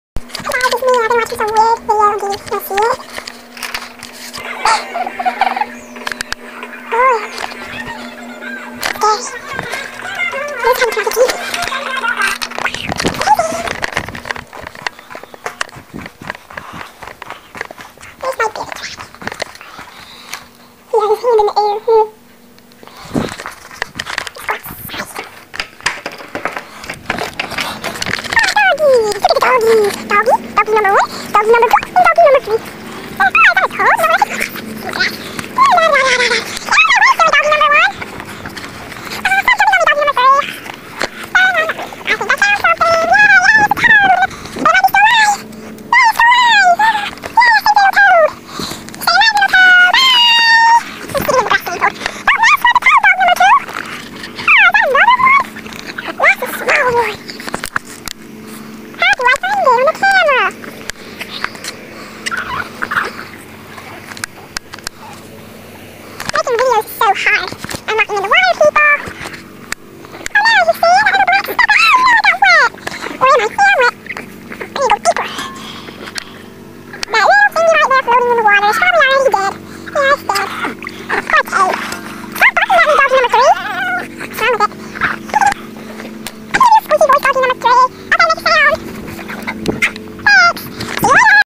Bob looks for Toads
Hi! My name is Bob and I'm looking for toads with my doggie friends!